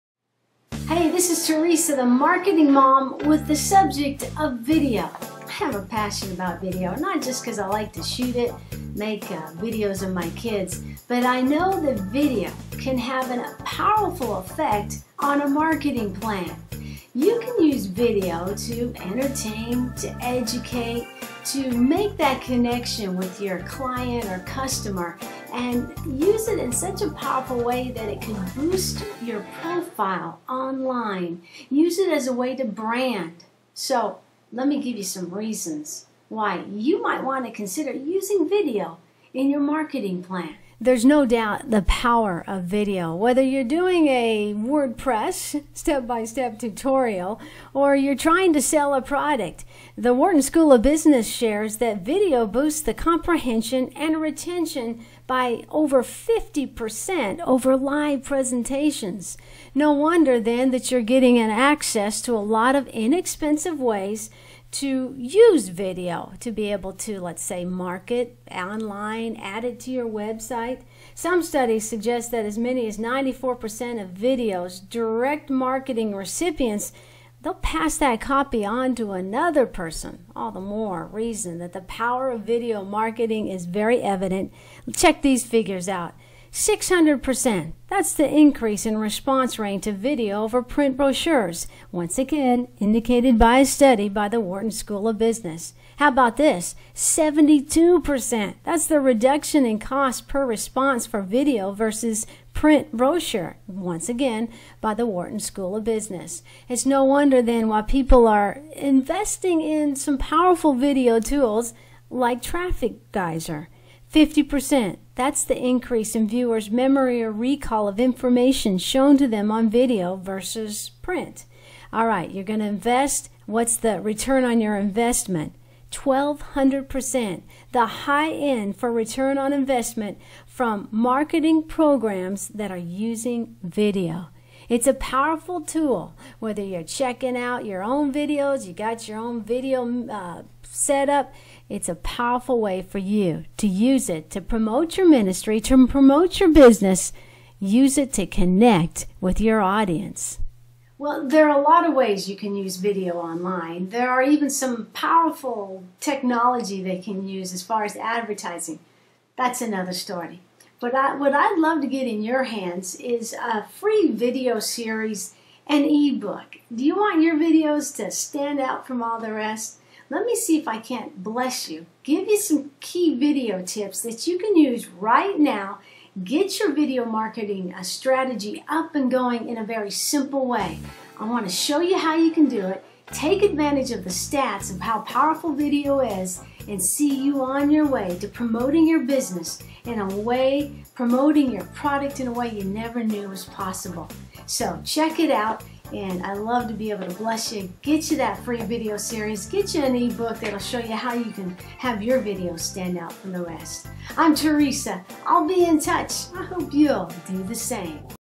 Marketing Christian Music With Video
Marketing Christian Music or your ministry can be advanced by using simple video marketing keys in your online promotion. Free Video Series teaching video ministry marketing.